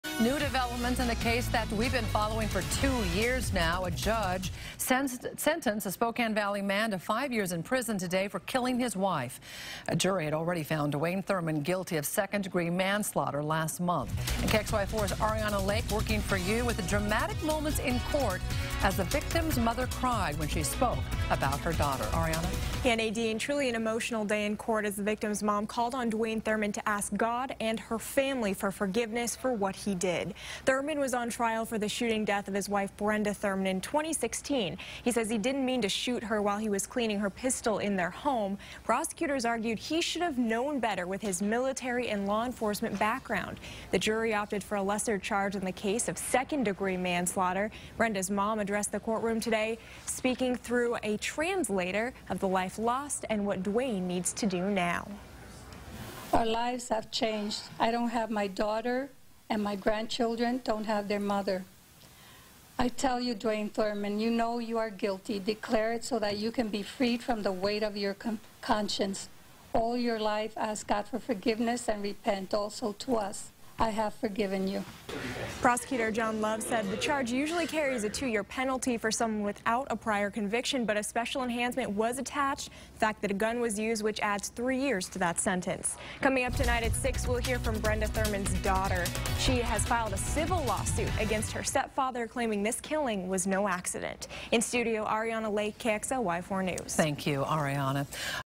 Thurman sentenced to 5 years in prison for 2016 shooting death of wife
Dwayne Thurman, 44, has been sentenced to 60 months, or 5 years, in prison for the 2nd degree manslaughter of his wife Brenda Thurman in 2016. KXLY4's Ariana Lake reports.